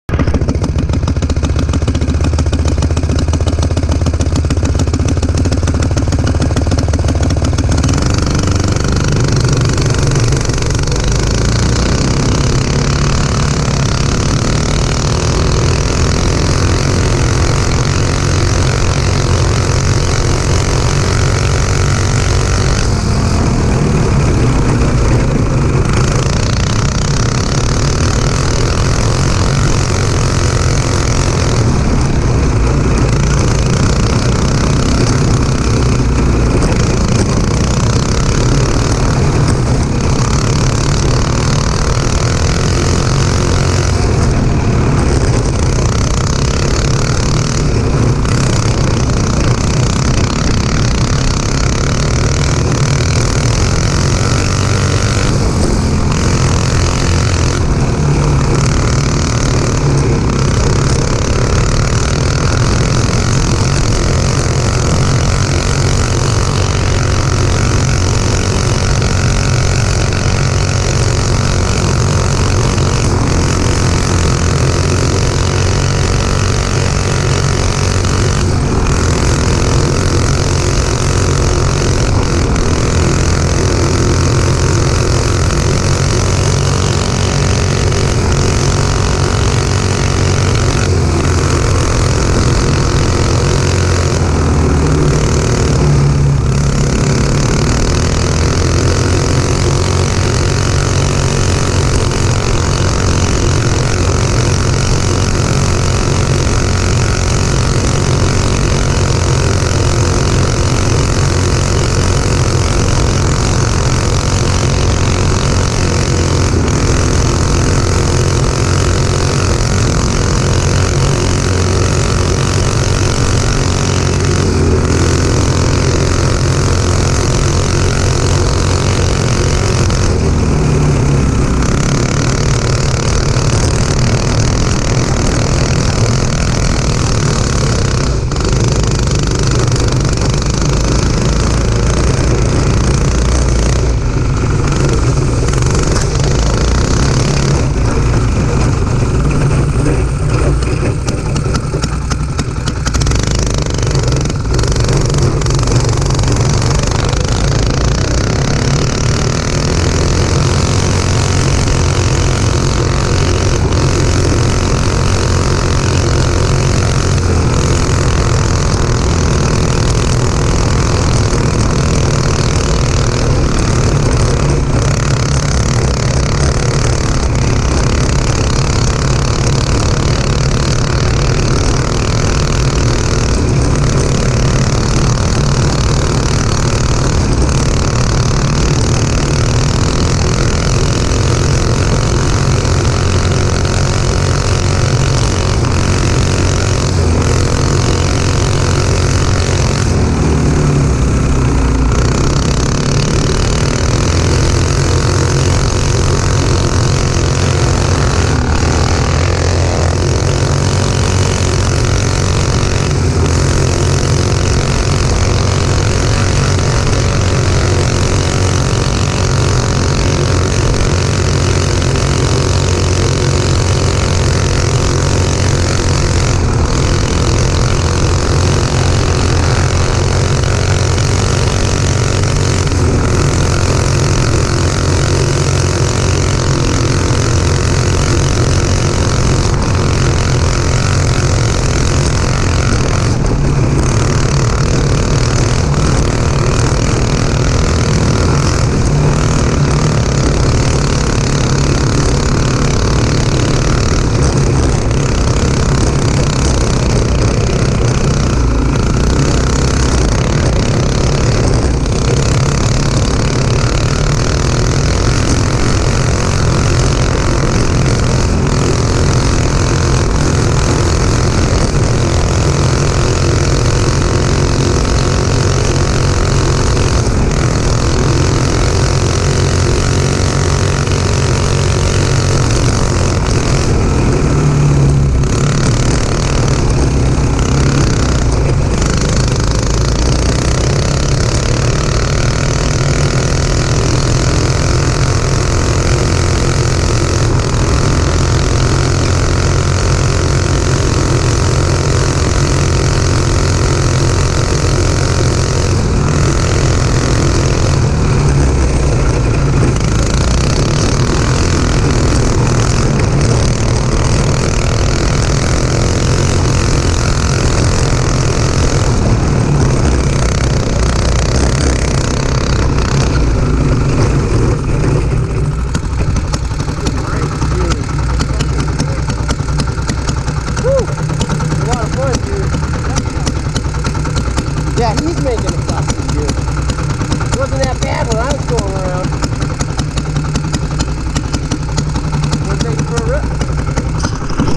Crazy final tune, Hell run, send it! Dust bowl!
Riding the kart with myself (Doug) and my buddies Andy and Joe.